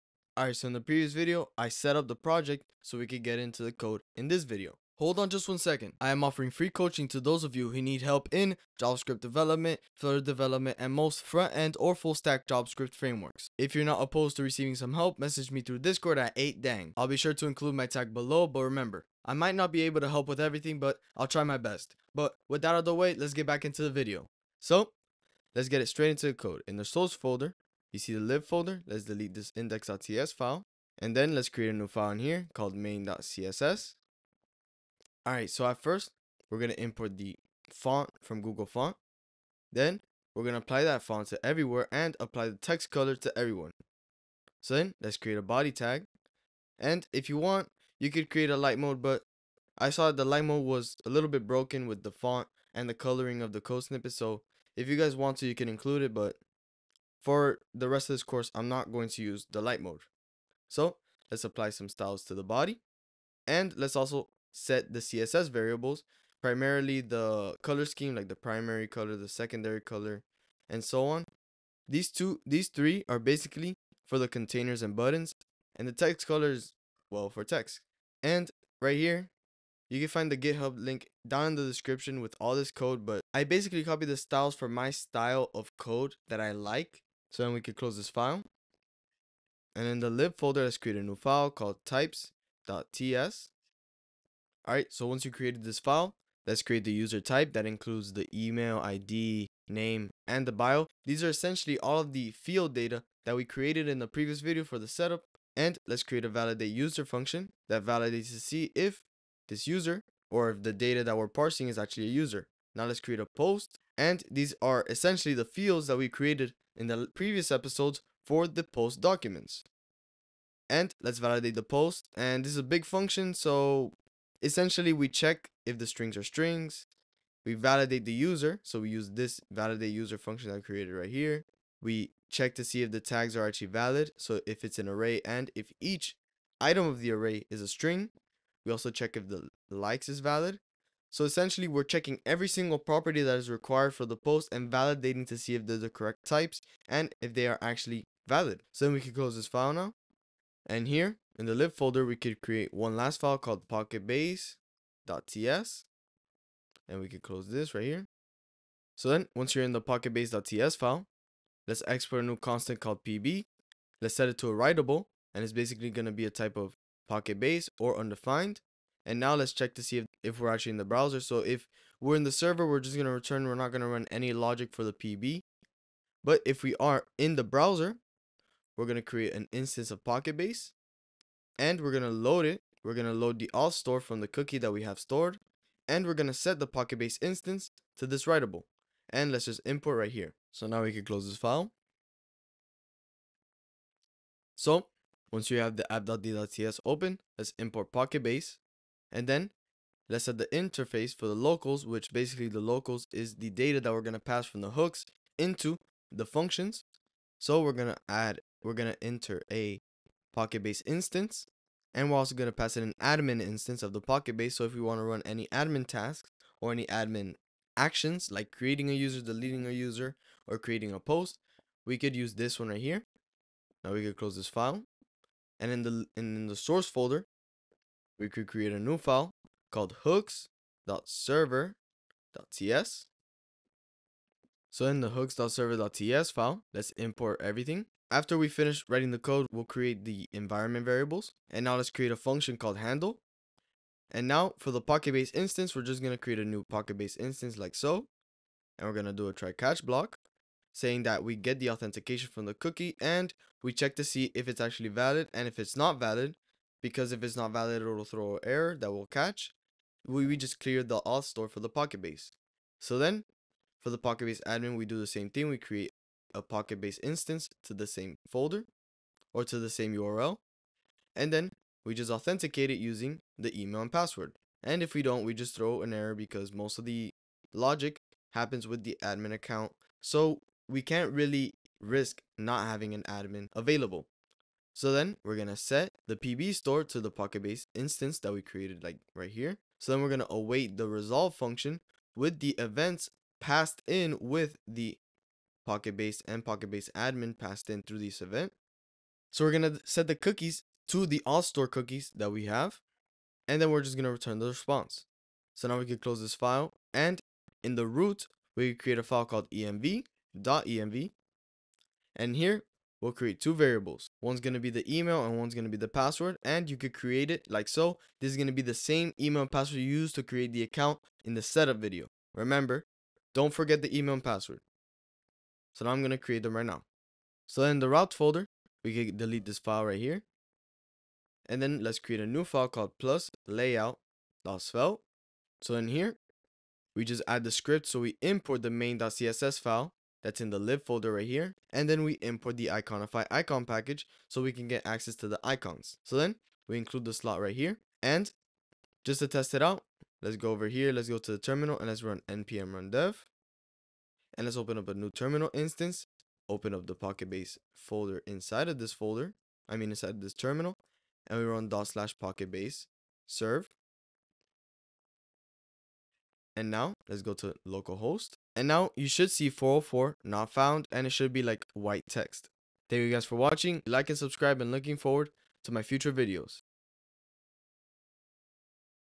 Create a Social Media Platform using SvelteKit and PocketBase - Auth Setup
Discord tag: 8dang

00:00 - Intro
00:04 - Coaching Plug
00:24 - Styles
01:34 - TS Types
02:31 - PocketBase Stuff
03:13 - Project Hooks
05:09 - Environment Variables
05:32 - Layout